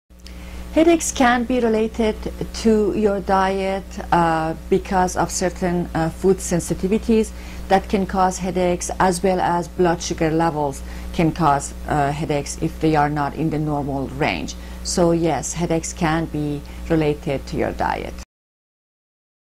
Can headaches be releated to diet: FtLauderdale Chiropractor
Headache pain treatment and therapy Dr. Maryam Jafarieh chiropractic physician
east Ft. Lauderdale Chiropractor on east commercial blvd. Dr. J on common cause and
cures for headache and migraine pain.